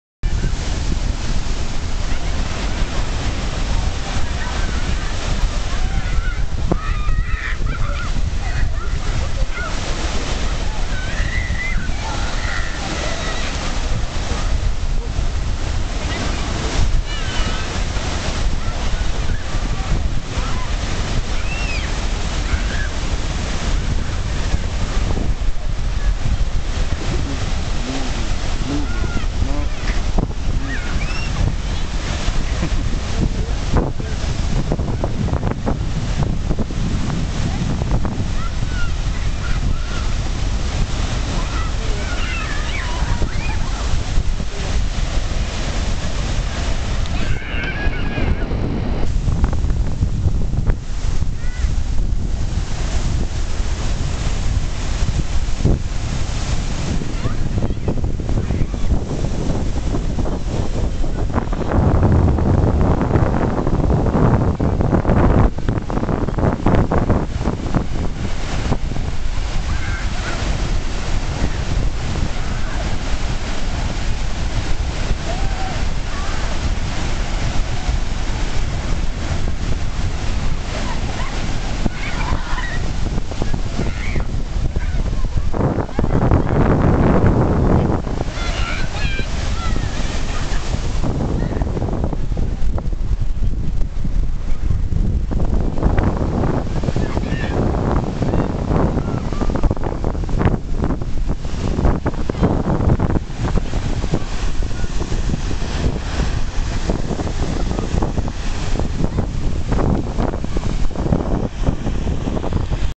Tulum Ruins, Tulum beach day trip.
Water was crystal, clear blue and waves were so fun!
The ruins were really cool; iguanas all over the place.
Tulum is about a 25 minute drive form where we stayed in Akumal.